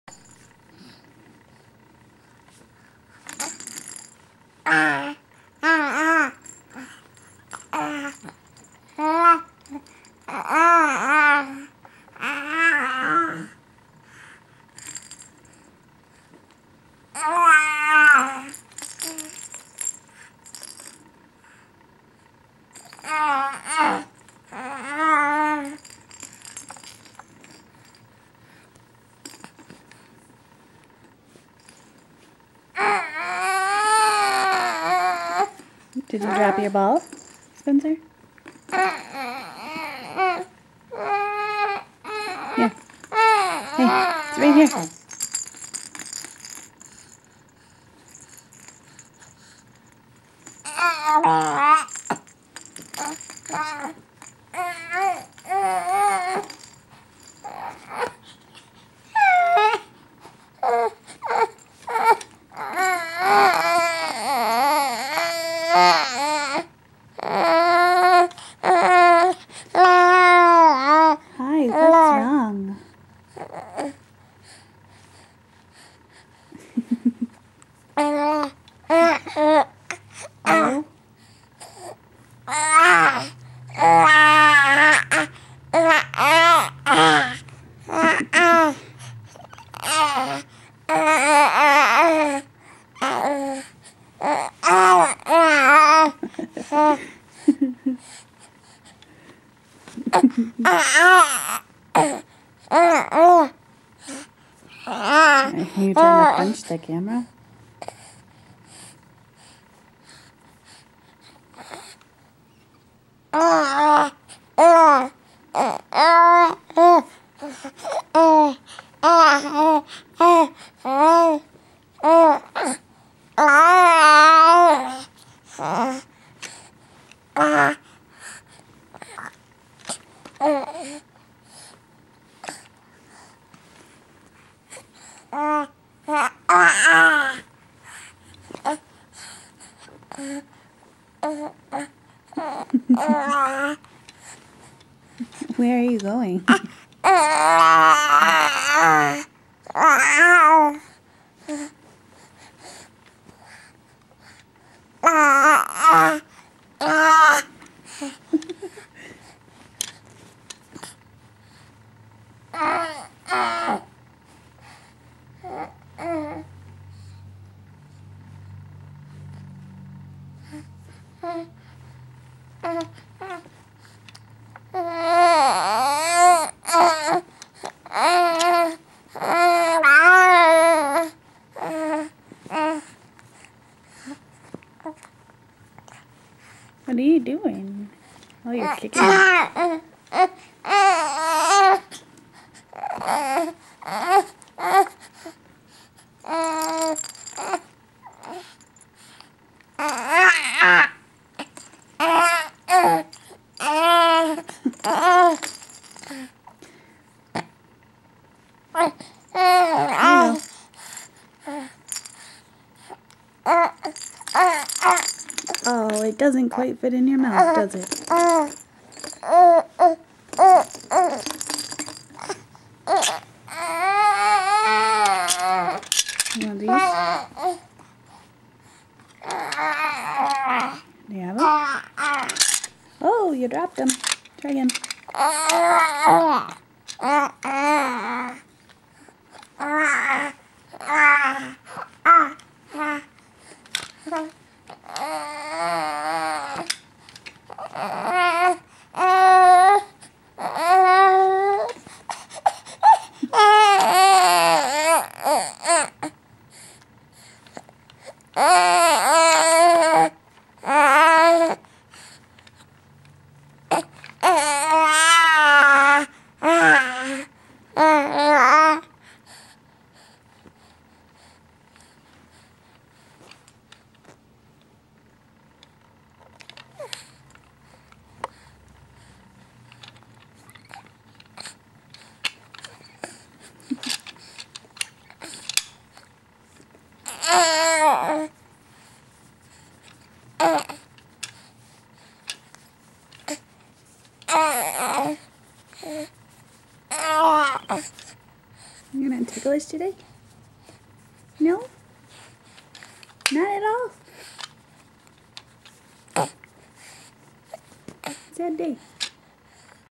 Baby Einstein Ball
Spencer likes to grunt at his toy. 15 weeks old.